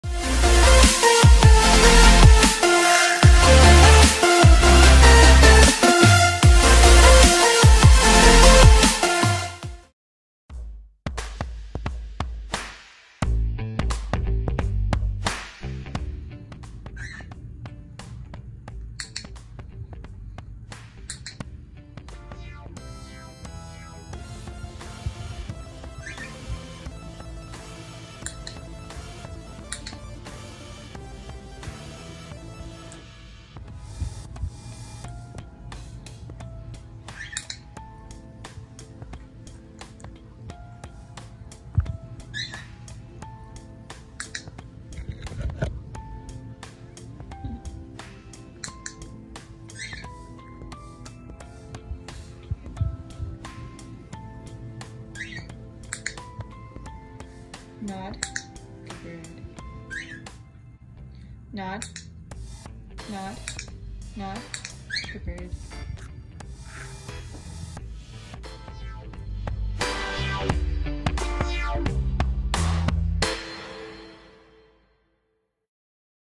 How To Teach Your Bird To Nod!
This is how I taught my bird to nod. This may not work for everyone, but it has worked for mine and others’ birds and can work for you too.